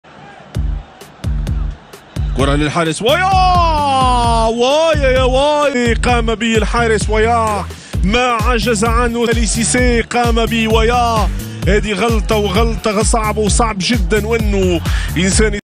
离大谱，惊天乌龙！门将爆射自家球门
在2022年2月的一场摩洛哥甲级比赛中，Rapide oued zem队的门将接后卫回传时没停好球，然后直接就来了一脚凌空爆射，打入一脚惊天乌龙。